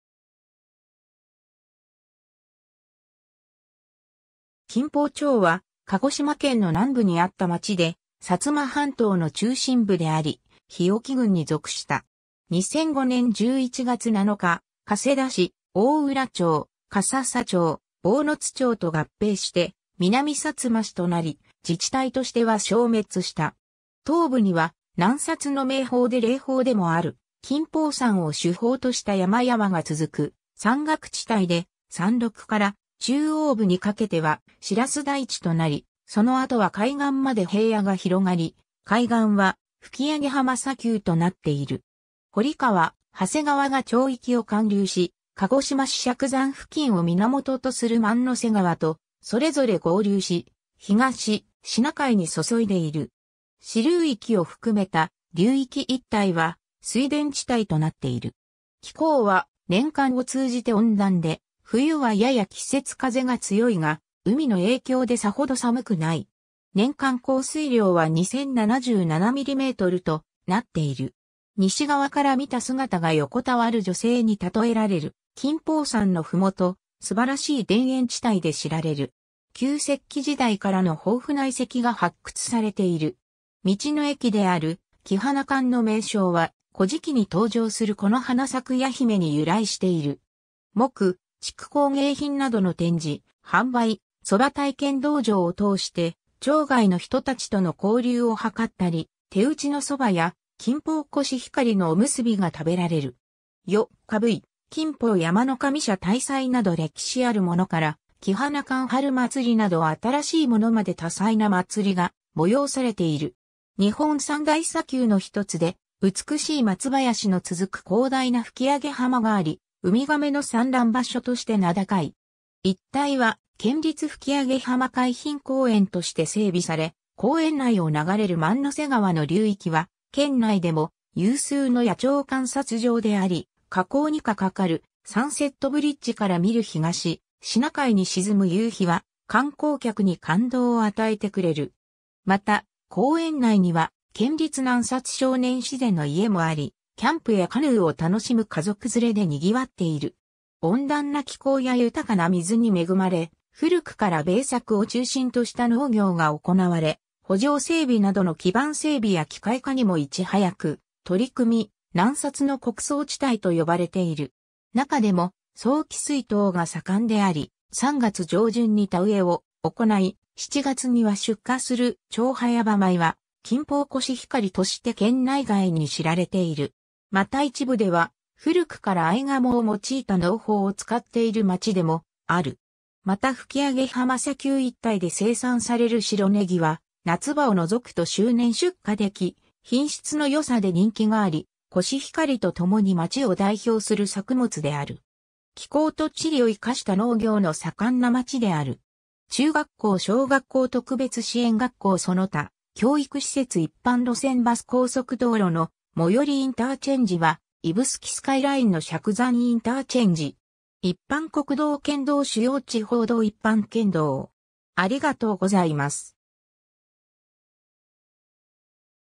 金峰町
金峰町（きんぽうちょう）は、鹿児島県の南部にあった町で薩摩半島の中心部であり、日置郡に属した。
 2005年11月7日、加世田市・大浦町・笠沙町・坊津町と合併して南さつま市となり、自治体としては消滅した。
 東部には、南薩の名峰で霊峰でもある金峰山（636m）を主峰とした山々が続く山岳地帯で、山麓から中央部にかけてはシラス台地となり、その後は海岸まで平野が広がり、海岸は吹上浜砂丘となっている。
堀川・長谷川が町域を貫流し、鹿児島市錫山付近を源とする万之瀬川とそれぞれ合流し、東シナ海に注いでいる。
支流域を含めた流域一帯は水田地帯となっている。
 気候は年間を通じて温暖で、冬はやや季節風が強いが、海の影響でさほど寒くない。
年間降水量は2077mmとなっている。
西側からみた姿が横たわる女性に例えられる金峰山の麓、素晴らしい田園地帯で知られる。
 旧石器時代からの豊富な遺跡が発掘されている。
道の駅である｢木花館｣の名称は古事記に登場する木花咲耶姫に由来している。
木・竹工芸品などの展示・販売、そば体験道場を通して町外の人たちとの交流を図ったり、手打ちのそばや、金峰コシヒカリのおむすびが食べられる。
よっかぶい(高橋十八度踊)、金峰山神社大祭など歴史あるものから、木花館春祭りなど新しいものまで多彩な祭りが催されている。
 日本三大砂丘の一つで、美しい松林の続く広大な吹上浜があり、海ガメの産卵場所として名高い。
一帯は県立吹上浜海浜公園として整備され、公園内を流れる万之瀬川の流域は県内でも有数の野鳥観察場であり、河口にか架かるサンセットブリッジから見る東シナ海に沈む夕日は観光客に感動を与えてくれる。
また、公園内には県立南薩少年自然の家もあり、キャンプやカヌーを楽しむ家族連れで賑わっている。
 （鹿児島県風土記より） 温暖な気候や豊かな水に恵まれ、古くから米作を中心とした農業が行われ、圃場整備などの基盤整備や機械化にもいち早く取り組み、「南薩の穀倉地帯」と呼ばれている。
なかでも早期水稲が盛んであり、3月上旬に田植えを行い、7月には出荷する超早場米は「金峰コシヒカリ」として県内外に知られている。
また一部では古くからアイガモを用いた農法を使っている町でもある。
また吹上浜砂丘一帯で生産される白ネギは、夏場を除くと周年出荷でき、品質の良さで人気があり、コシヒカリとともに町を代表する作物である。
 気候と地理を活かした農業の盛んな町である。
 中学校 小学校 特別支援学校 その他教育施設 一般路線バス 高速道路の最寄りインターチェンジは指宿スカイラインの錫山インターチェンジ。
 一般国道 県道 主要地方道 一般県道